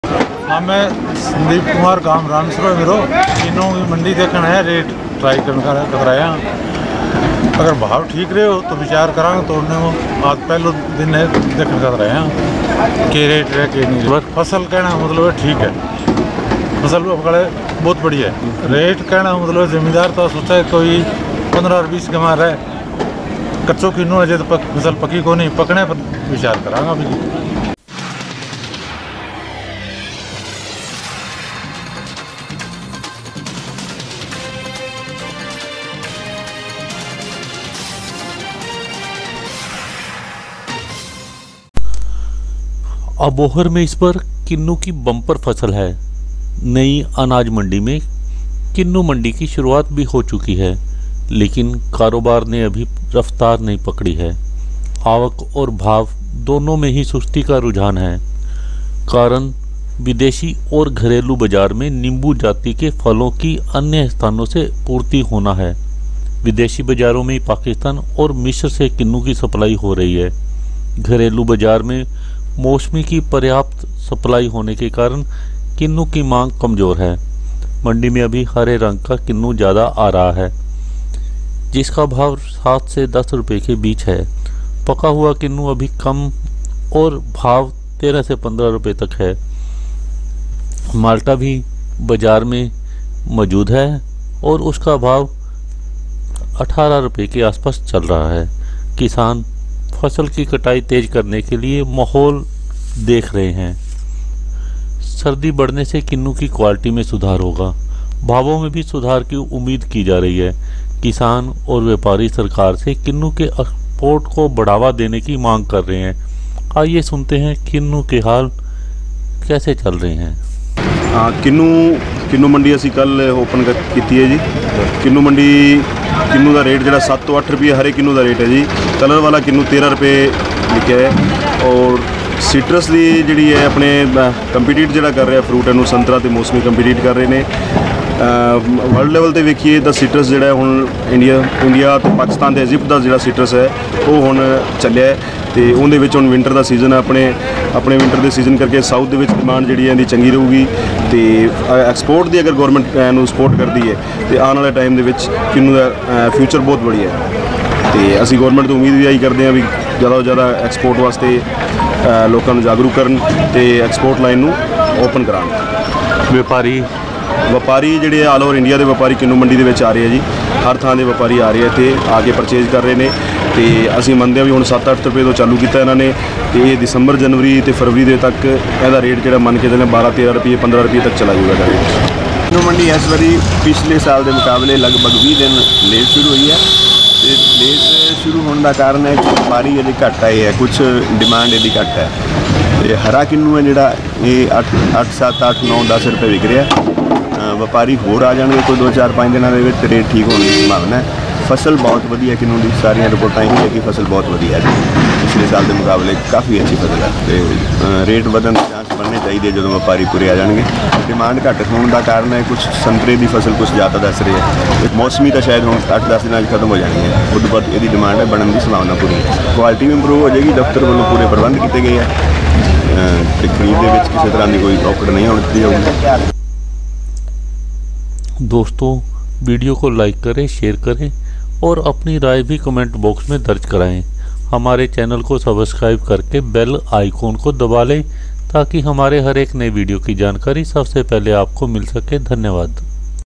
Kinnow Mandi Started In Abohar / Moderate Demand Of Kinnow In Abohar Kinnow Mandi
Kinnow Mandi Started In Abohar Punjab
 Moderate Demand  Of Kinnow In Abohar Mandi Punjab
अबोहर की किन्नू मंडी की अभी सुस्त चल रही है सफ्तार

This video is about Kinnow season started in Abohar. There is bumper kinnow crop in Abohar this season. Kinnow Mandi Abohar has begun in New Grain Market Abohar premises. Beginning of kinnow mandi Abohar is delayed by at least twenty days. Rates of kinnow in Abohar are between Rs seven to ten.